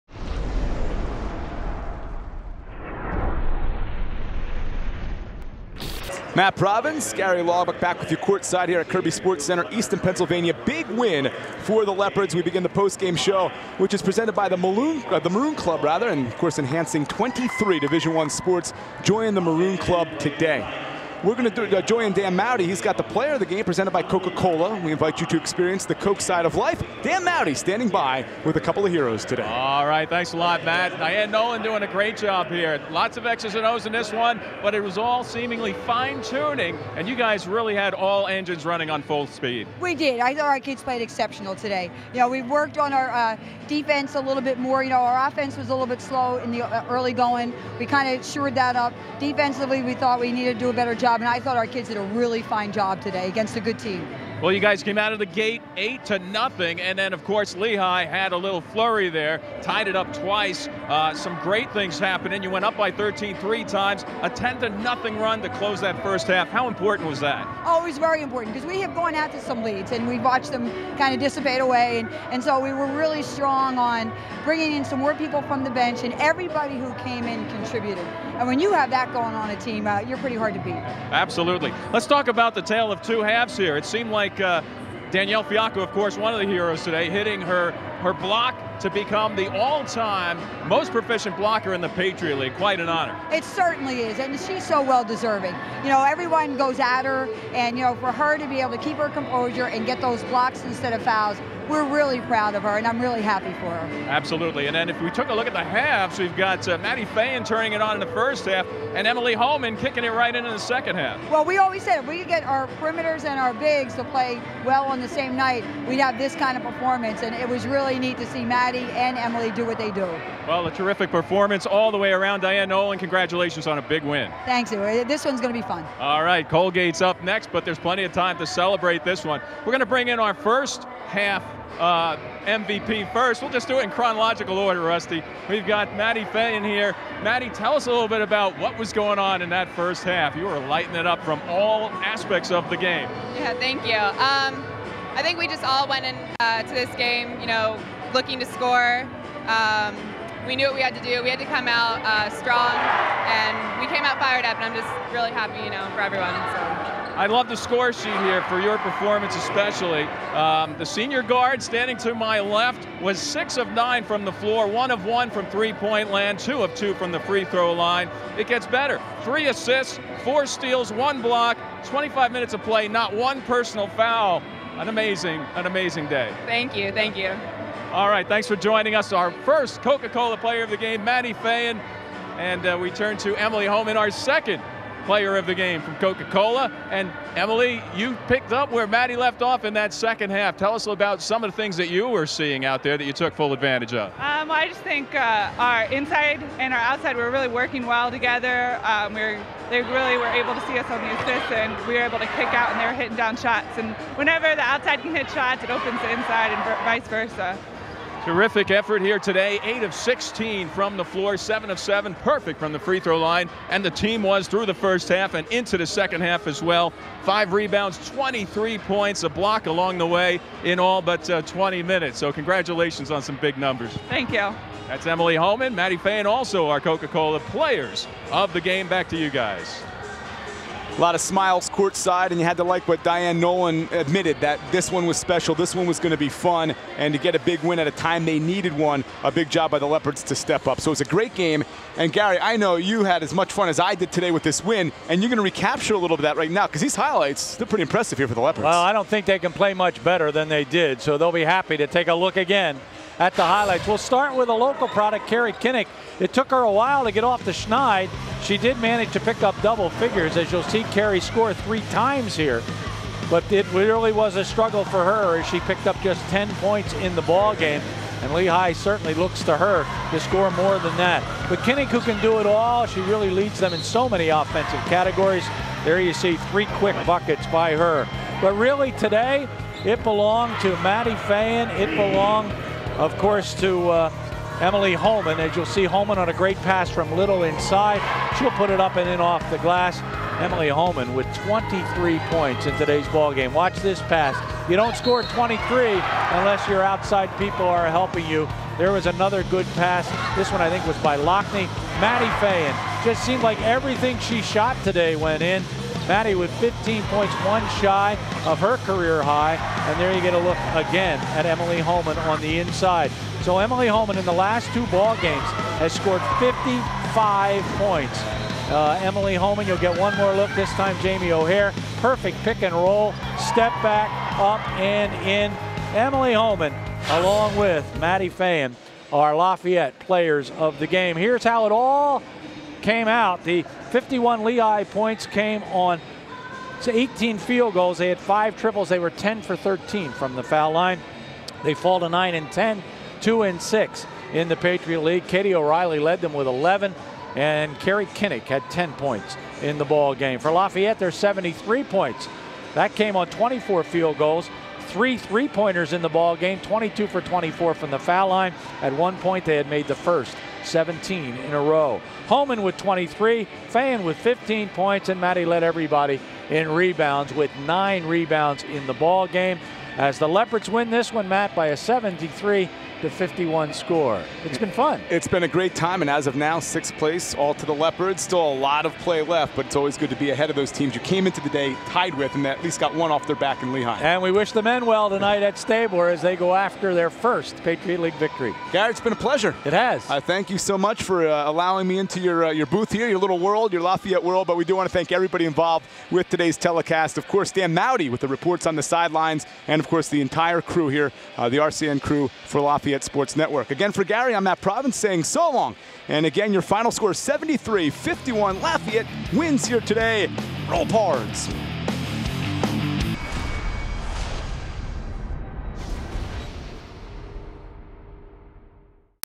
Postgame: Lafayette Women's Basketball vs Lehigh
Highlights and interviews following Lafayette's 73-51 win over rival Lehigh.